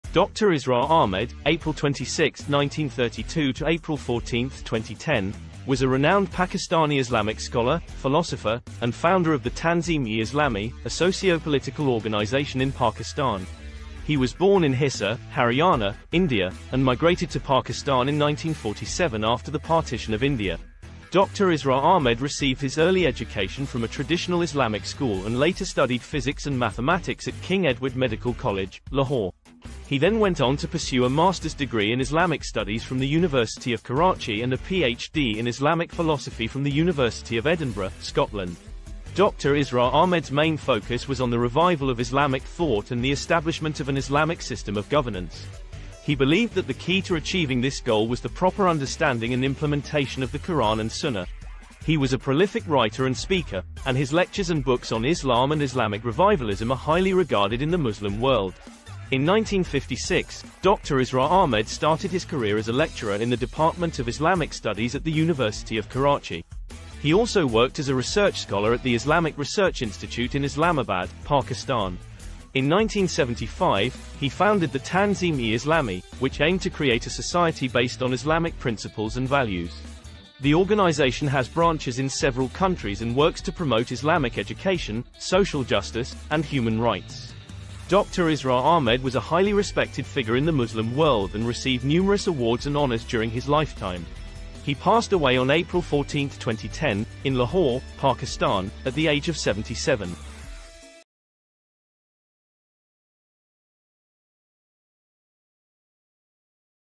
Biography of Doc Israr Ahmed
Dr. Israr Ahmed (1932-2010) was a Pakistani Islamic scholar, speaker, and founder of the Islamic organization Tanzeem-e-Islami